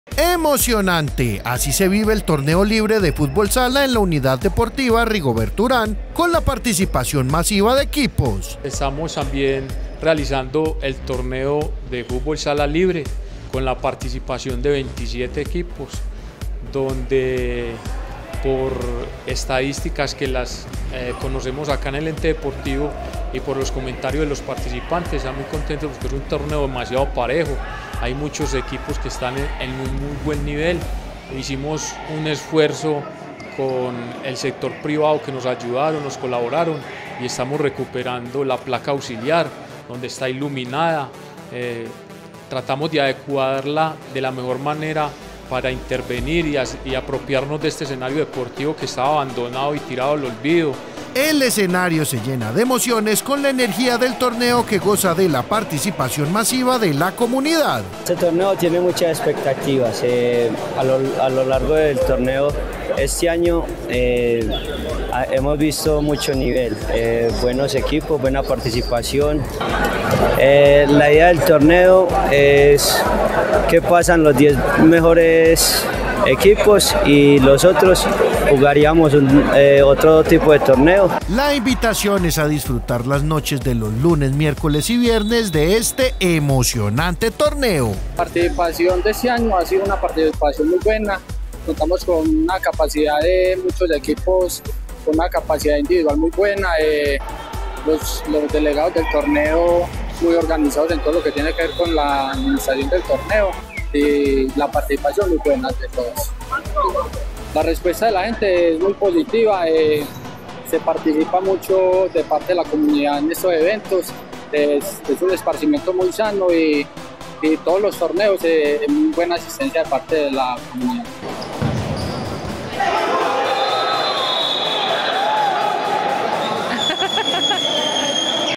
Torneo de fútbol sala libre
En la unidad deportiva Rigoberto Urán se lleva a cabo, el torneo de fútbol sala libre, con una nutrida participación y un nivel bastante competitivo, veamos cómo se desarrolla este emocionante certamen